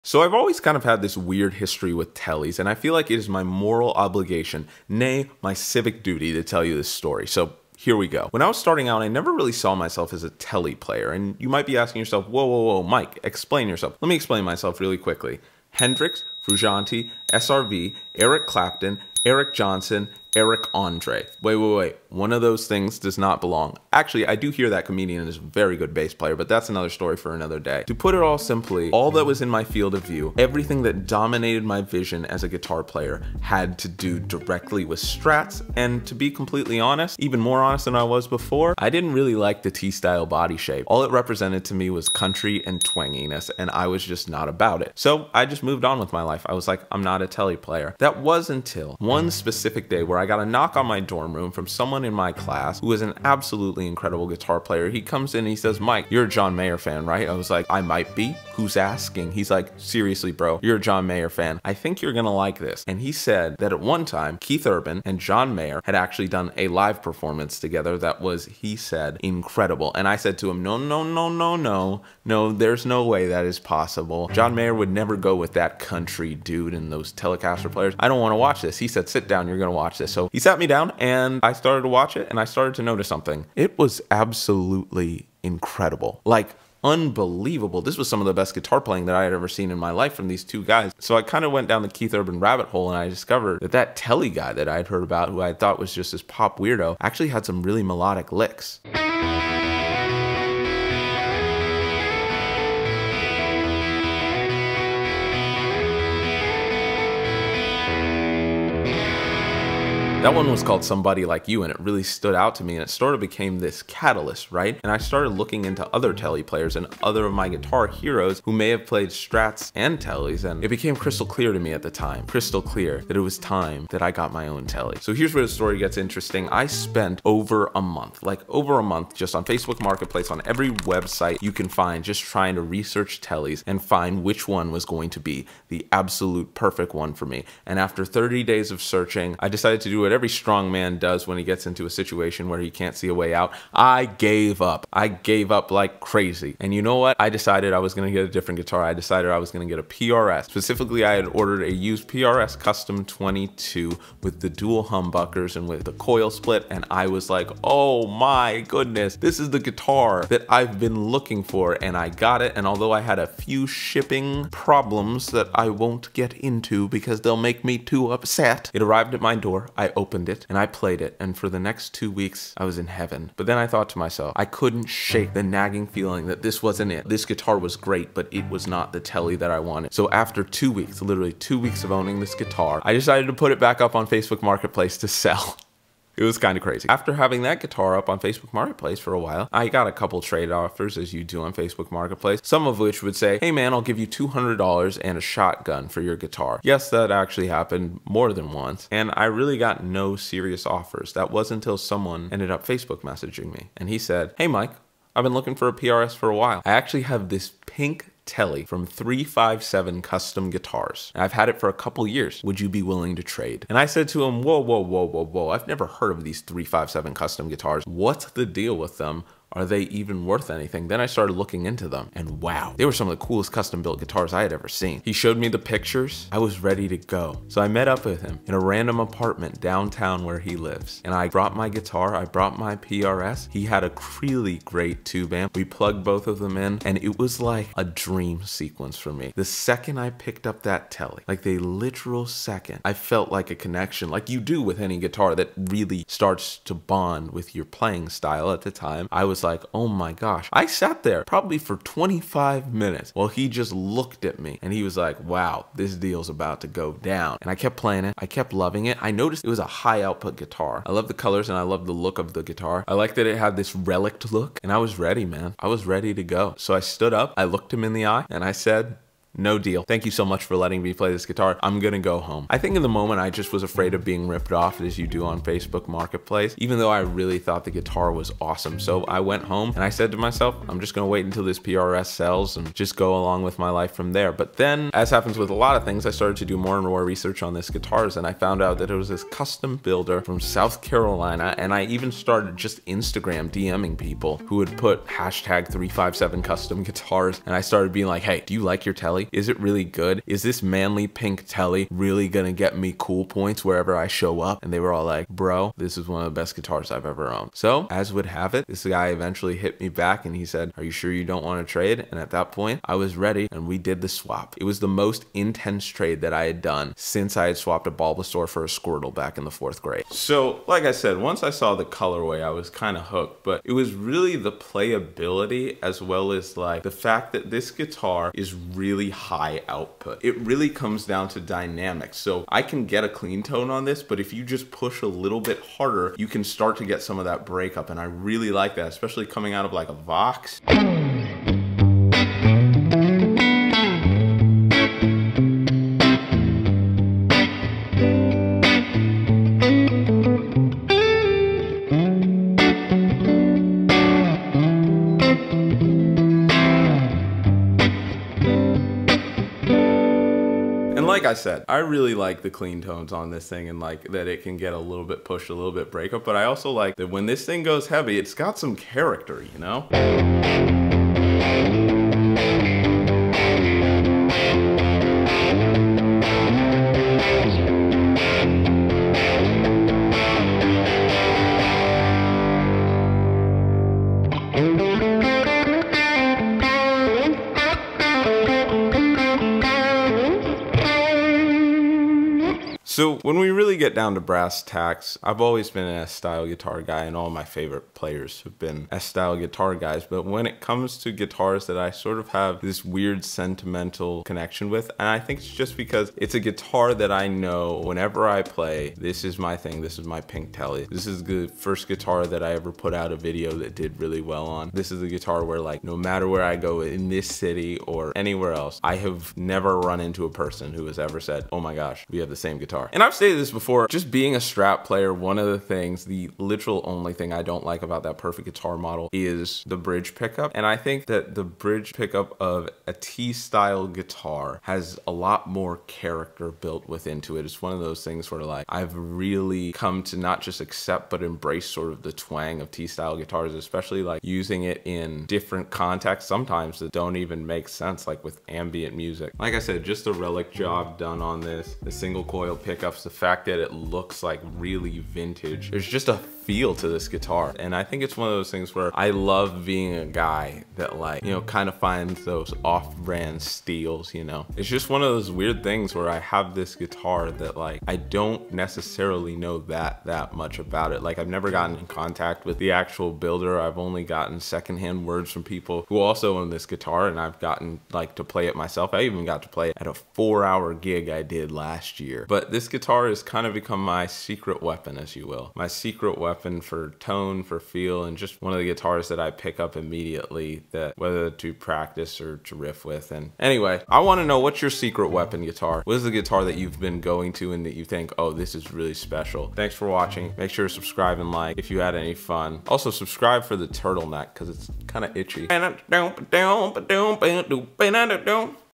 This is NOT a Fender Telecaster
This is the story I guess. Because I think that the question that I get asked the most is about my pink guitar so I decided I really wanted to final tell you guys about it! Hope you enjoy!